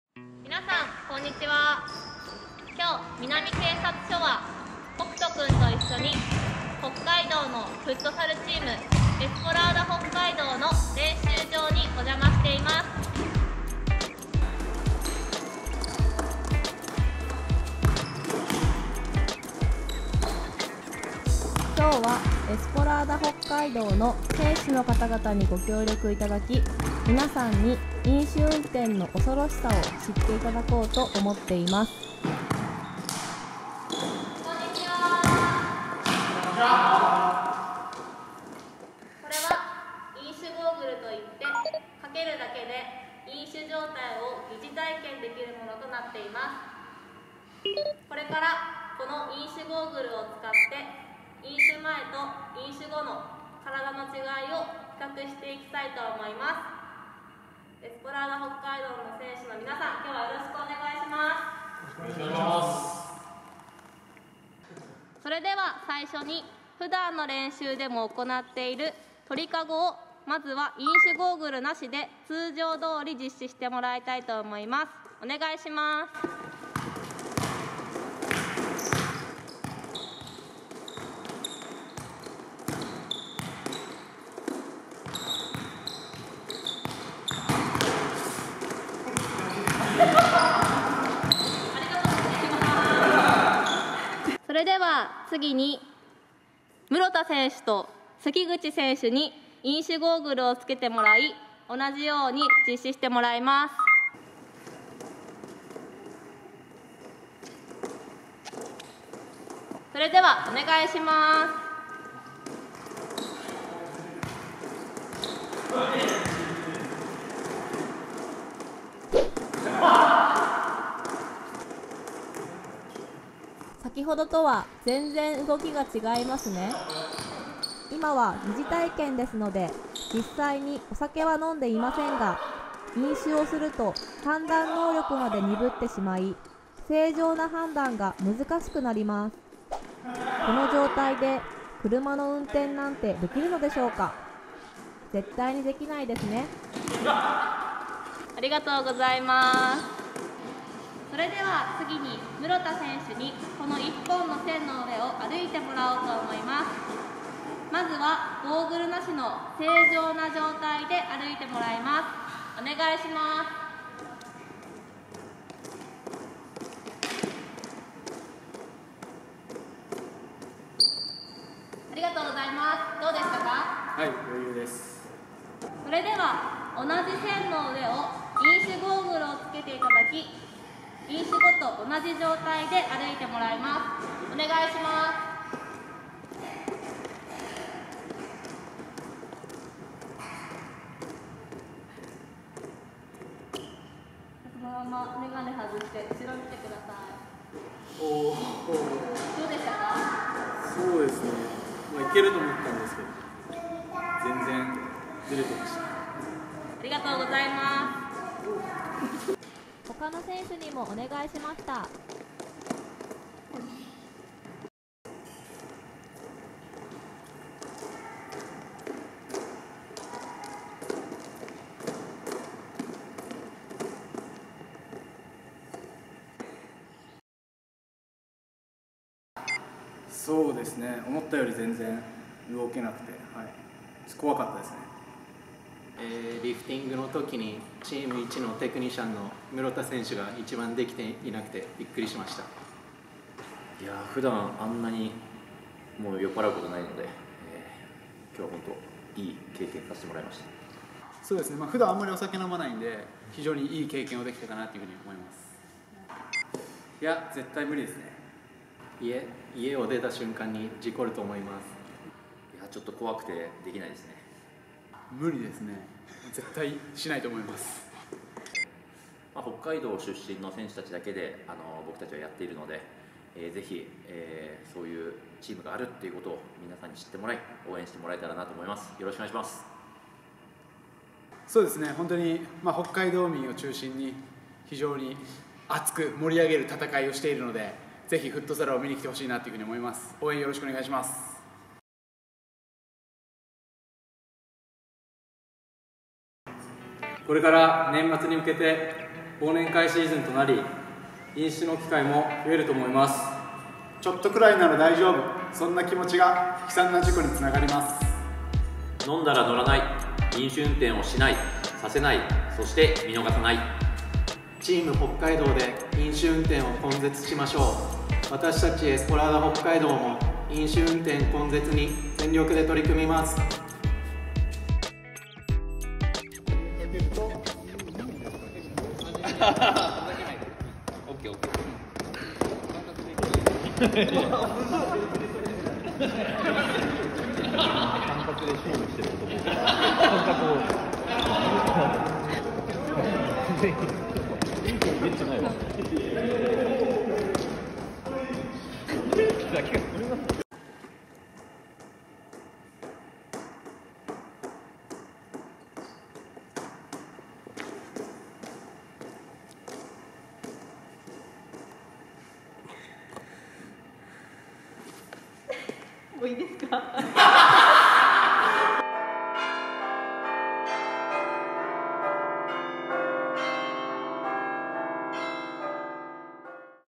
札幌方面南警察署✕エスポラーダ北海道✕飲酒運転根絶メッセージ動画
・飲酒運転根絶に向けたメッセージ
・飲酒後の酔いの状態を視覚体験できる飲酒体験ゴーグルを装着した上でさまざまな動きをしてもらい、飲酒前後における体の動きの変化を比較したもの。